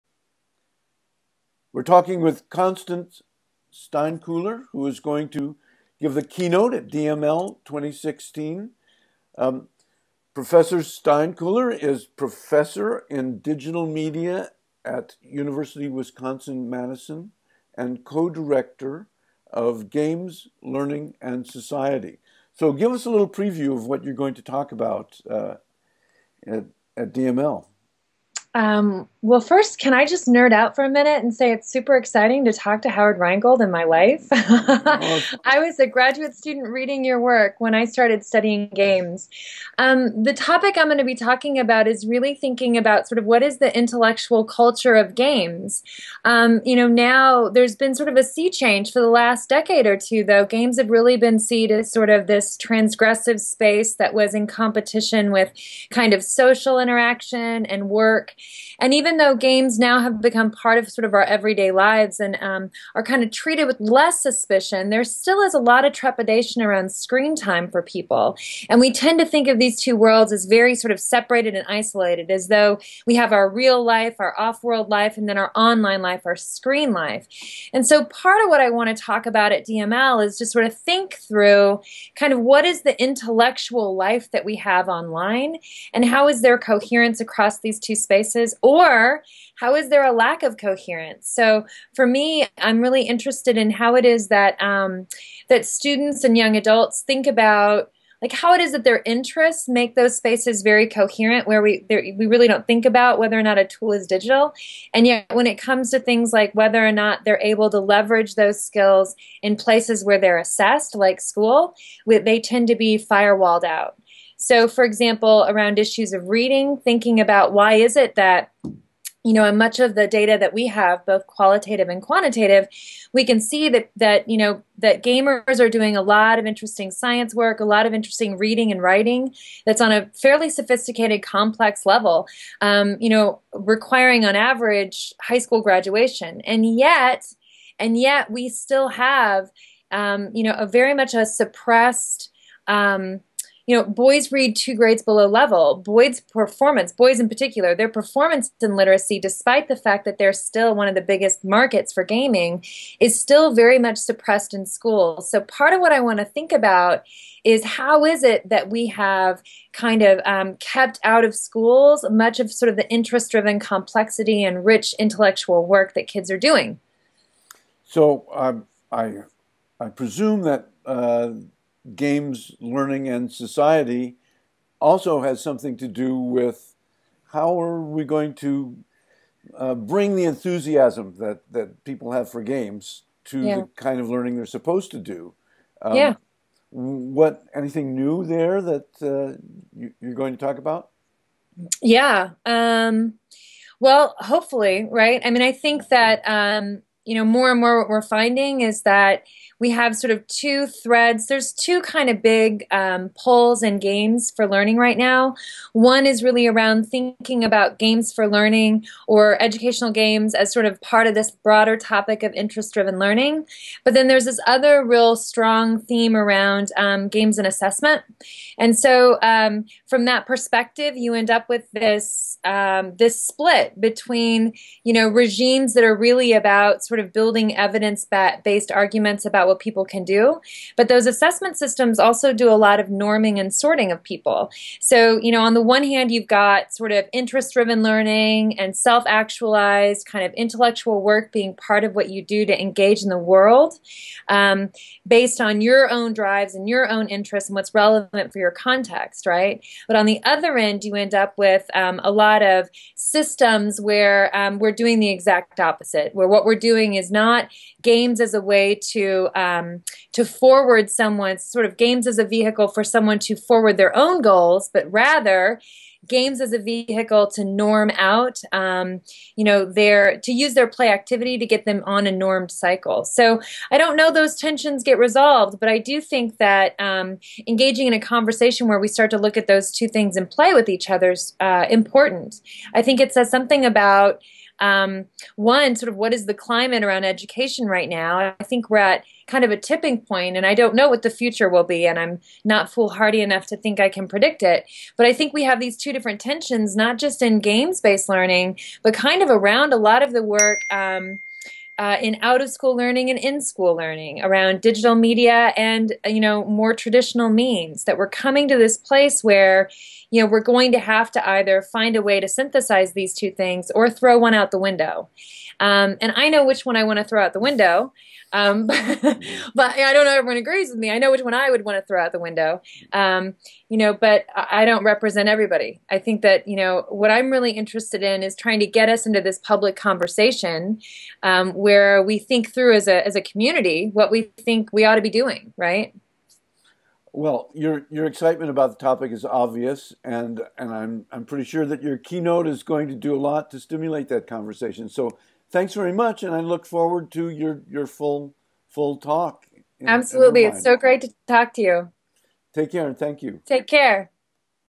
DML2016 Keynote Speaker Interview - Constance Steinkuehler
The 2016 DML Conference keynote speaker, Constance Steinkuehler, gives a preview of what she will talk about at the DML Conference in this interview with Howard Rheingold. Visit dml2016.dmlhub.net for more information on the DML Conference and to sign up.